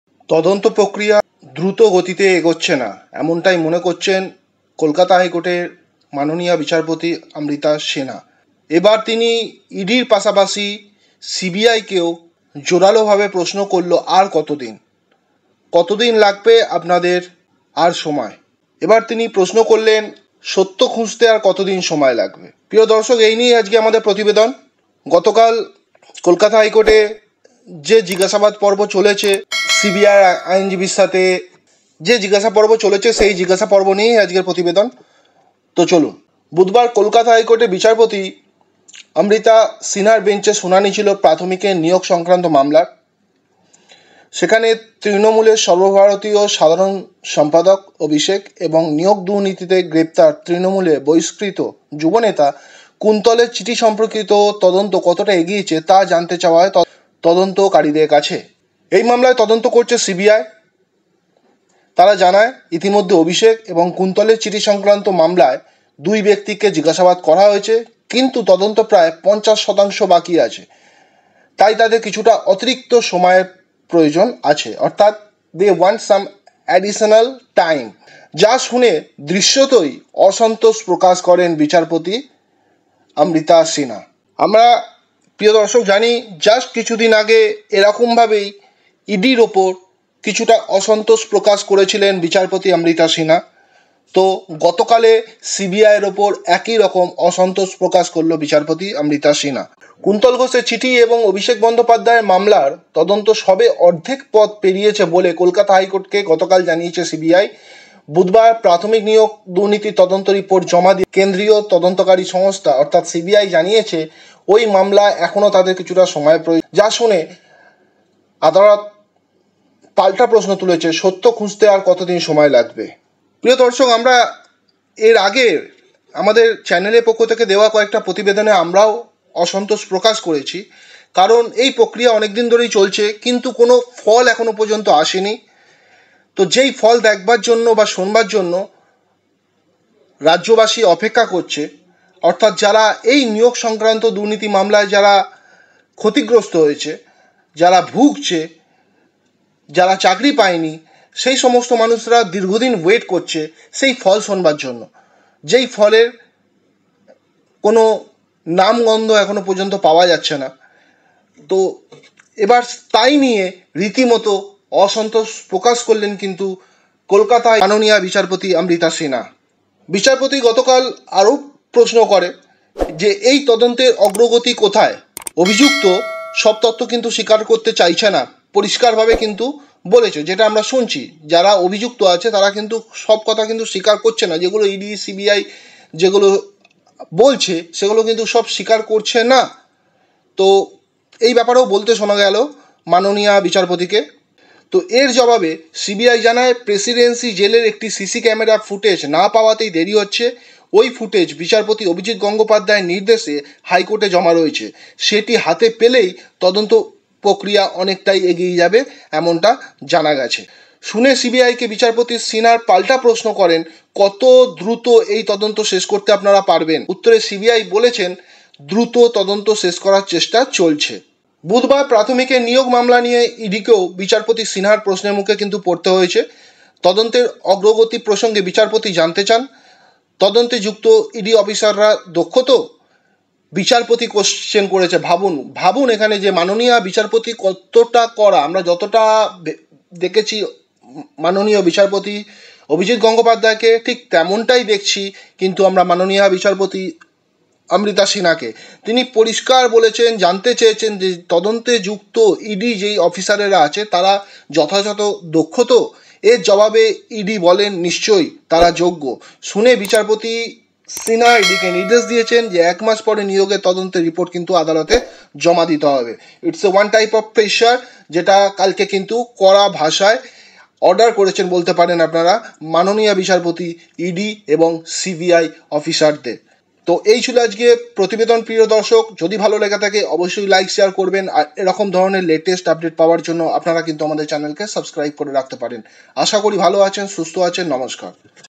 WB Recruitment Scam News : কেন্দ্রীয় তদন্তকারী সংস্থার উপর অসন্তোষ প্রকাশ করলেন বিচারপতি
It is a completely competitive exam and government job related education channel.